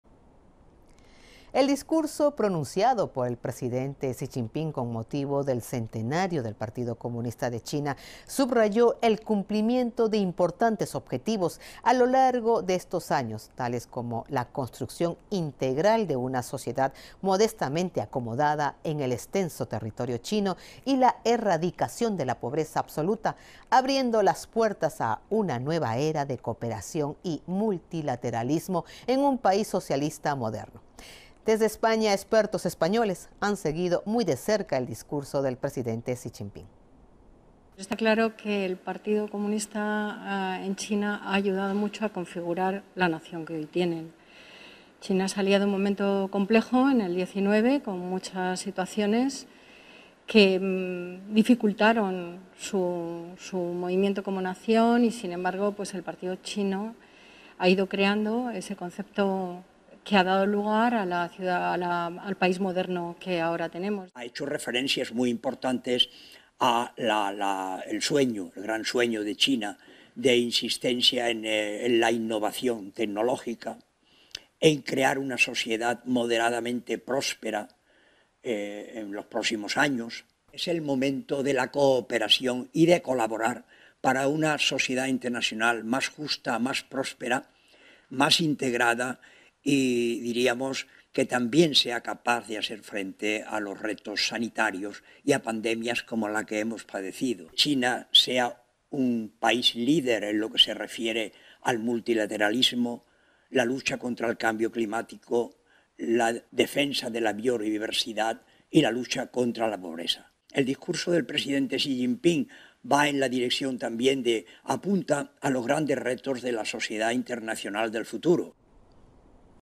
Expertos españoles destacan la importancia de la cooperación como clave en el futuro de China
El discurso pronunciado por el presidente Xi Jinping con motivo del centenario del Partido Comunista de China subrayó el cumplimiento de importantes objetivos a lo largo de estos años, tales como la construcción integral de una sociedad modestamente acomodada en en el extenso territorio chino o la erradicación de la pobreza absoluta y abrió las puertas a una nueva era de cooperación y multilateralismo, en un país socialista moderno. Desde España expertos españoles han seguido muy de cerca el discurso del presidente.